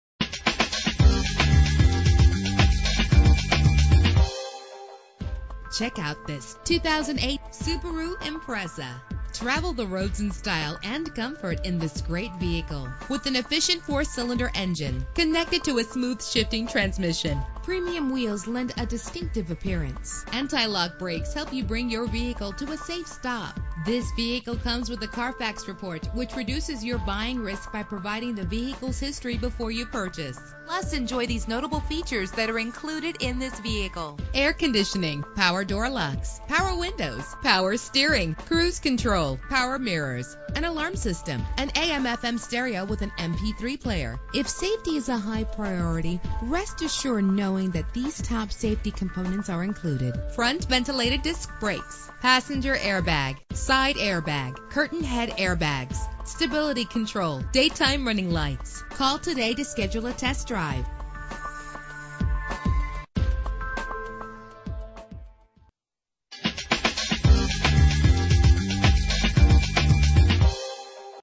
2008 Subaru Impreza - Stamford CT
Year: 2008
Make: Subaru
Model: Impreza
Trim: WRX
Engine: 2.5 liter H4 16 valve
Transmission:
Color: Silver
Mileage: 44381
Address:
198 Baxter Ave